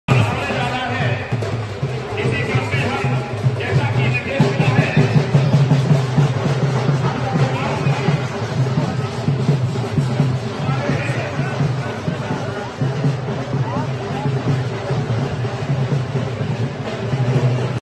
कन्नौज मे अखिलेश यादव से मिलने पहुंचे पार्टी के बड़े नेता
Video from जनसदेश टाइम